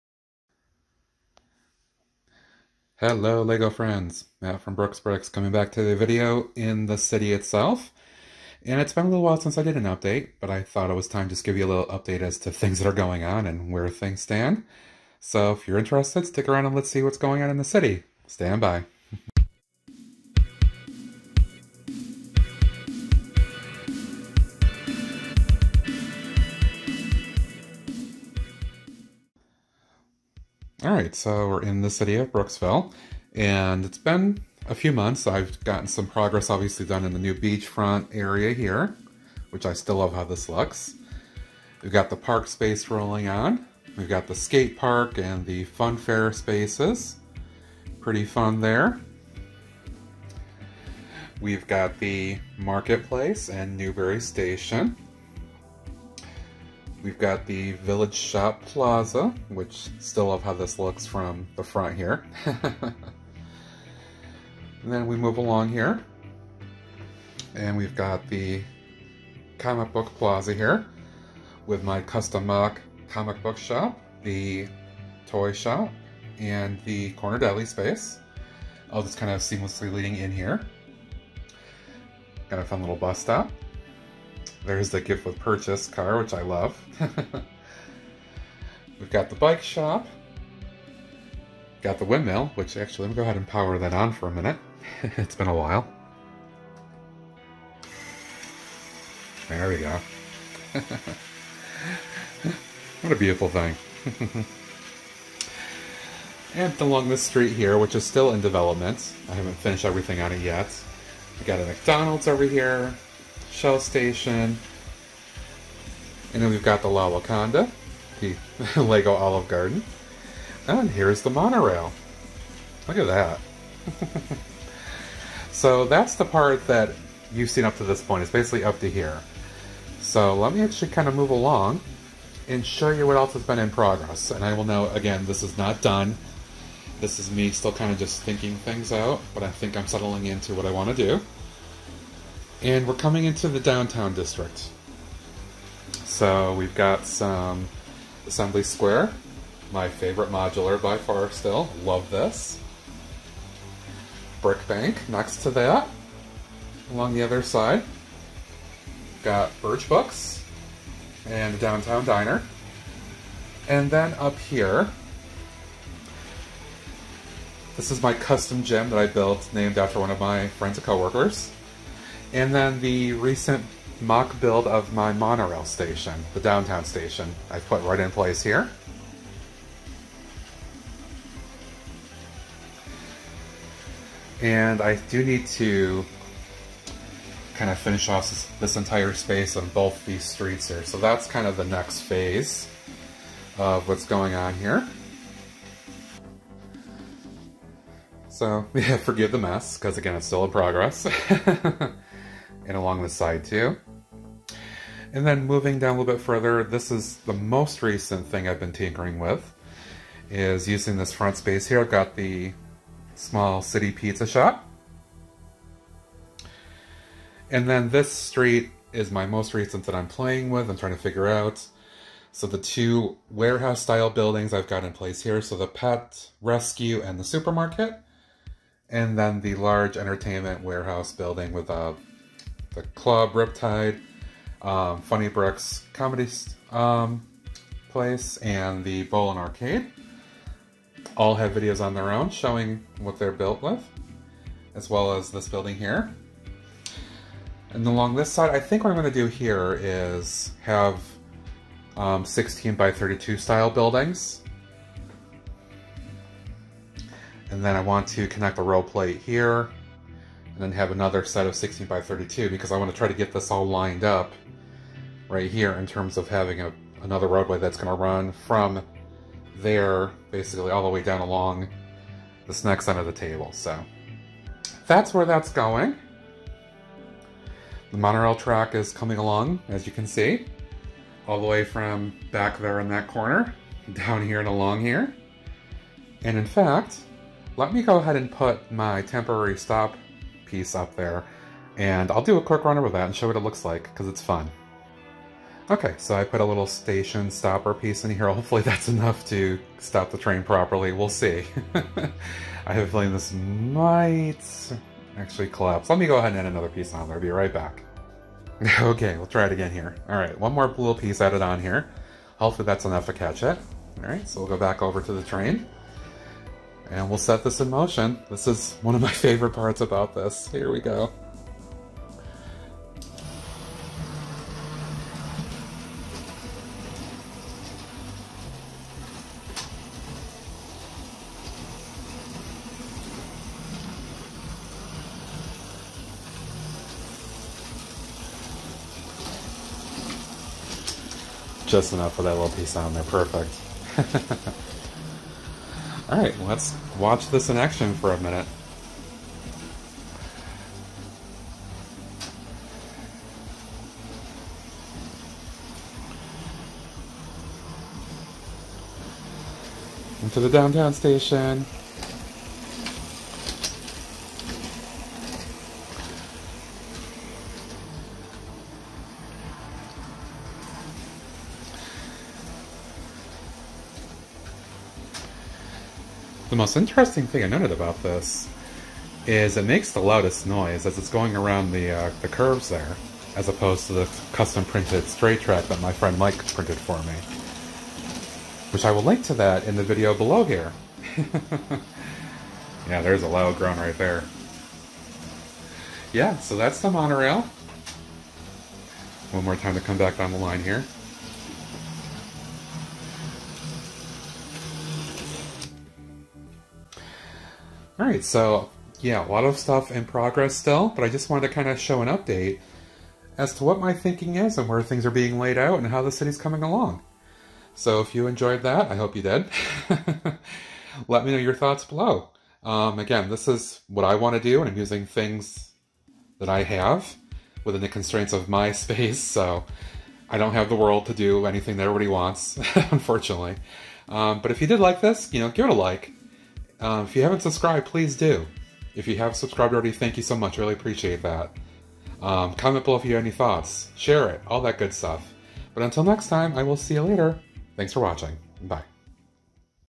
Brooksville City Update: 01-26-2021 -- Downtown District (In-Progress!)
In this latest Brooksville City Update, I review work done so far, and reveal the current development of the Downtown District, still a work in progress!  As a fun bonus, we can ride along with the monorail too!